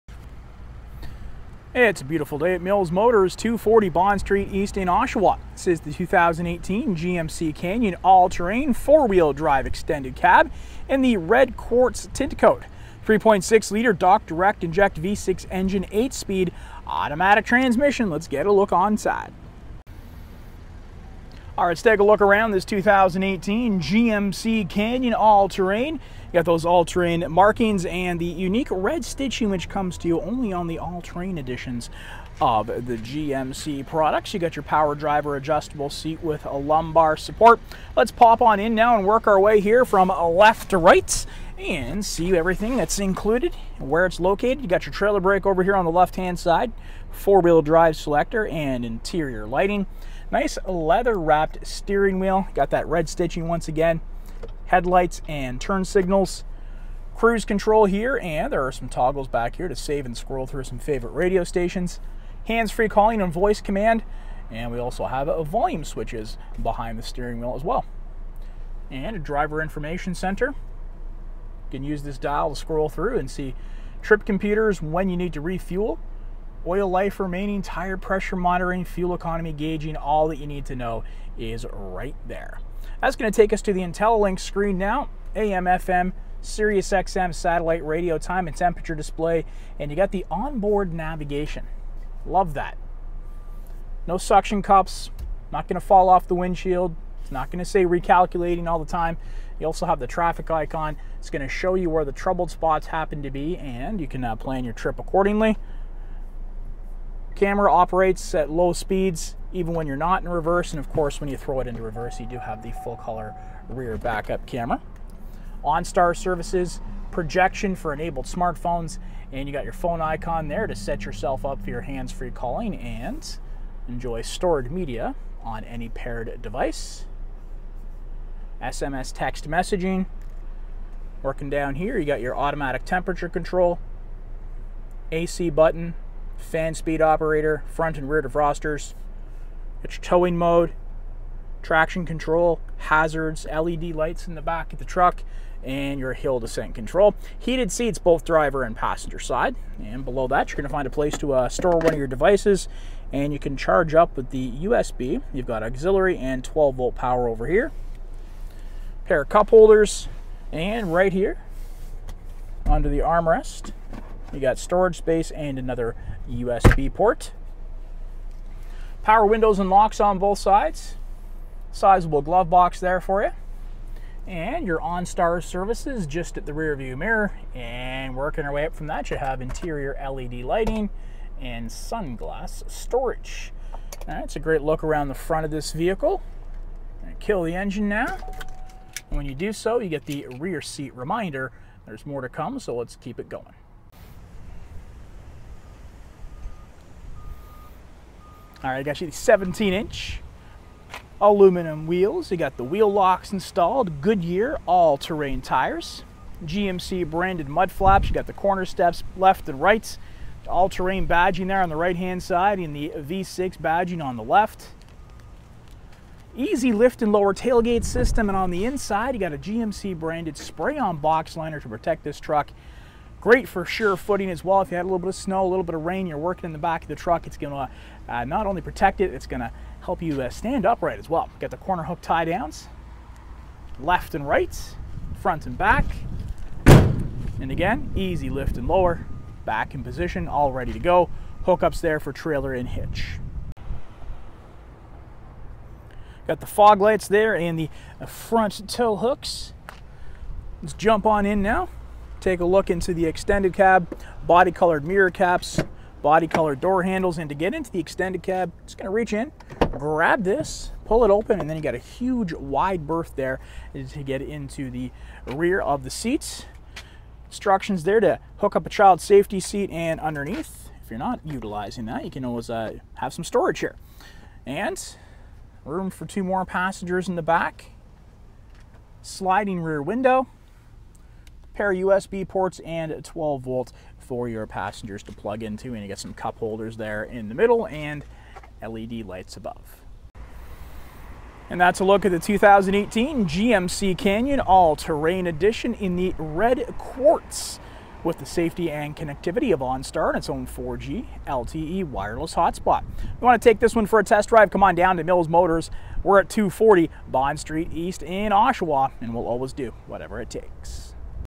2018 GMC Canyon All Terrain Extended Cab V6 Engine Spray On Liner Red Oshawa ON Stock #180217
To see more vehicles like this visit

Oshawa Ontario
L1H 7L3

This is a 2018 GMC Canyon with Automatic transmission Red Quartz Tintcoat color and Jet Black interior color.  This video is recorded and uploaded by cDemo Mobile Inspector